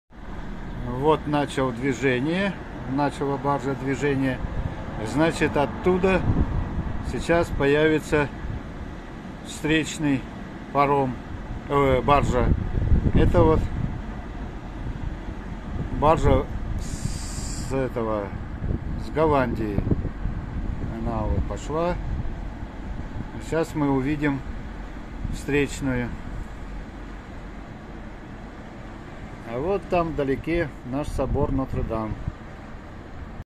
BARGES COASTING ON THE RIVER CANAL L'ESCAUT BELGIUM ...
The term "cabotage" comes from the French "cabotage", and the origins of the French word come from the Spanish "cabo", which means "cape". Historically, cabotage meant sailing without access to the open sea, from cape to cape. In this way, the first ships of the ancient Egyptians, Greeks and Romans moved.
Термин «каботаж» произошел от французского «cabotage», а истоки французского слова произошли от испанского «cabo», что означает «мыс». Исторически каботаж означал плавание без выхода в открытое моря, от мыса к мысу. Таким способом передвигались первые судоходы древних египтян, греков и римлян.